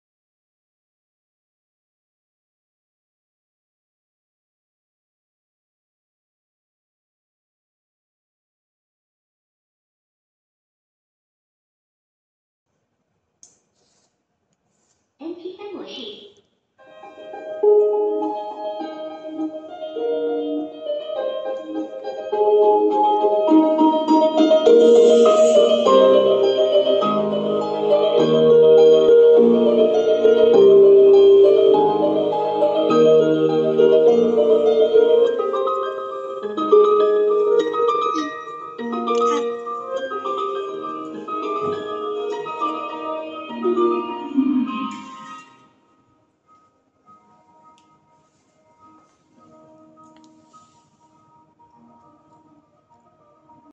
KITO Less Panel | Sound Insulation Testing
Test:
1. Ambient Sound: 41db
2. Incoming Sound ( Music from MP3) : 95db
3.put the sound equipment into foam ceramic box
Reflected Sound: 49.4db
4. Foam Ceramic material
Absorbed Sound: 45.6db

Green Energy Saving Building Material